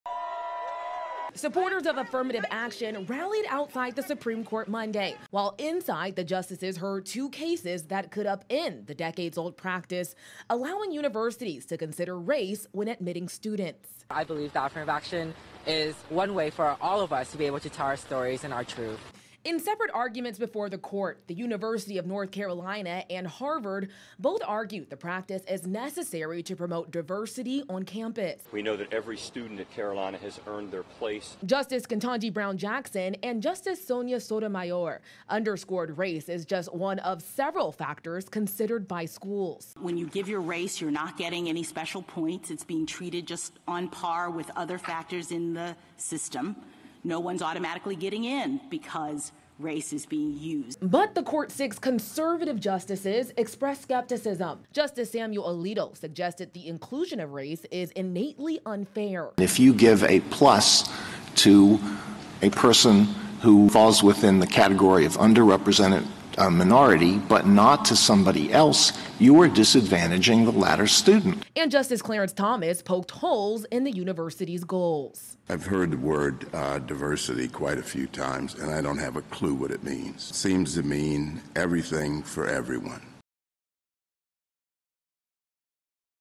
Supreme Court hears cases on affirmative action in schools
Raquel Martin reports from our DC Newsroom. (Oct. 31, 2022)